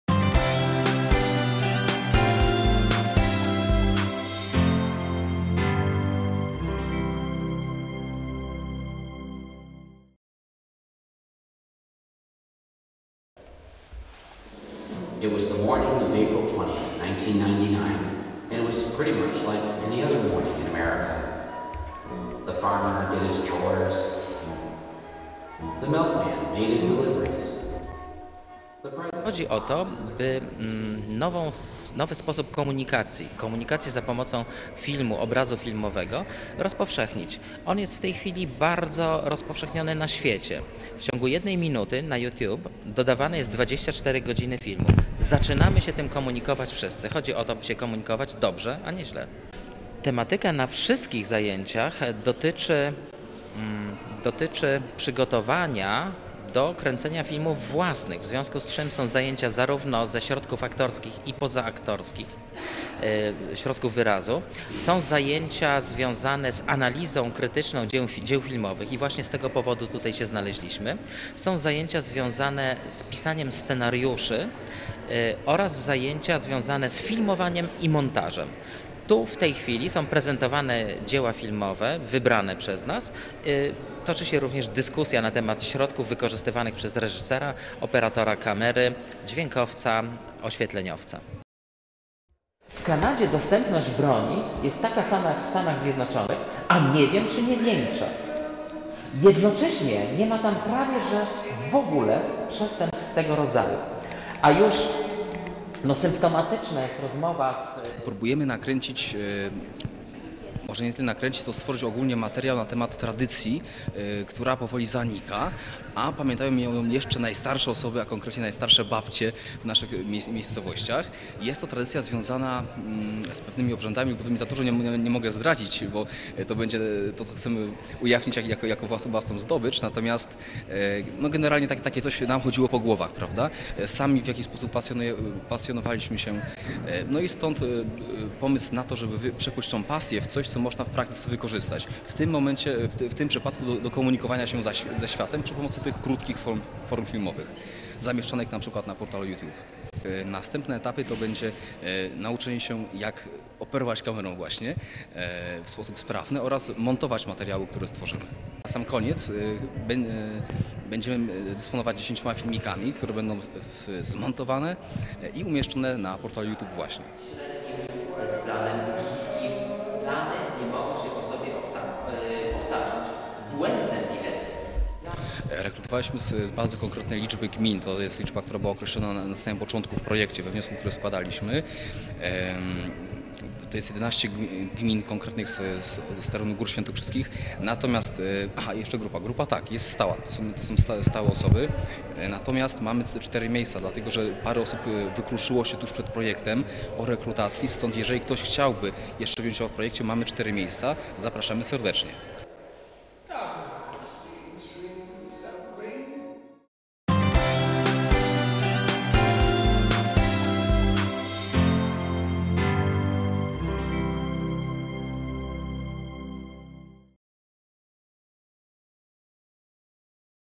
"Kocham Kino" - warsztaty filmowe w Ciekotach - 18.12.2011r.
W Szklanym Domu w Ciekotach odbyły się zajęcia w ramach projektu "Kocham kino" realizowanego przez Klub 3D - Debata, Decyzja, Działanie - z Bielin. Uczestnicy warsztatów obejrzeli i przeanalizowali filmy: "Kontrakt rysownika" oraz "Zabawy z bronią"